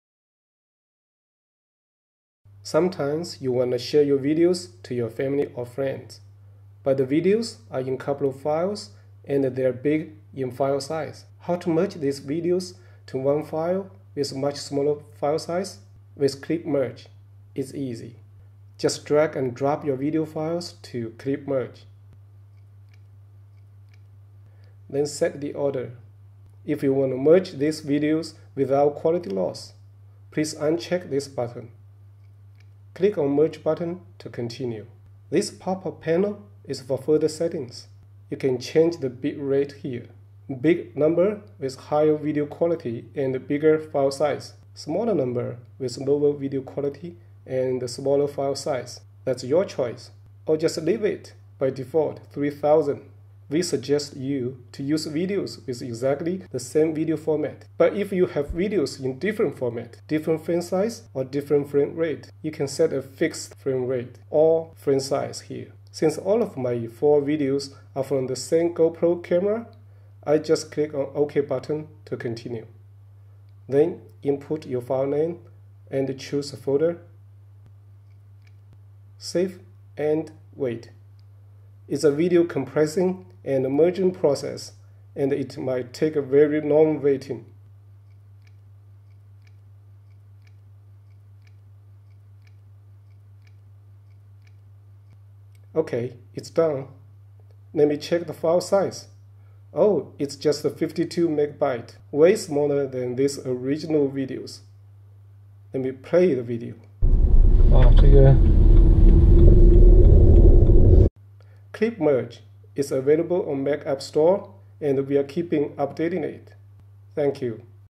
How to join videos into one file and reduce video file size
Sometimes you wanna share your videos to your family or friends. But the videos are in couple of files and they are big in file size. How to merge these videos to one file with much smaller file size? With Clipmerge, it’s easy.

Just drag and drop your video files to Clipmerge, then set the order. If you wanna merge the videos without quality loss, please uncheck this button.Click on Merge button to continue. This popup panel is for further settings. You can change the bitrate here. Bigger number with higher video quality and bigger file size. Smaller number with lower video quality and smaller file size. That’s your choice. Or just leave it by default 3000. Mostly you can reduce 5 times of the file size without losing video quality.

We suggest you to use videos with exactly same video format. But if you have videos in different format, different frame size or different frame rate, you can set a fixed frame size and frame rate here.

Since all of my 4 videos are from the same Gopro camera, I just click on OK button to continue. Then input your file name and choose a folder, Save and wait. It’s a video compressing and merging process and It might take a very long waiting. Ok, it’s done, let me check the file size. Oh, it’s just 52MB, way smaller than these original videos. Let me play the video.

Clipmerge is available on Mac App Store and we’re keeping updating it. Thank you.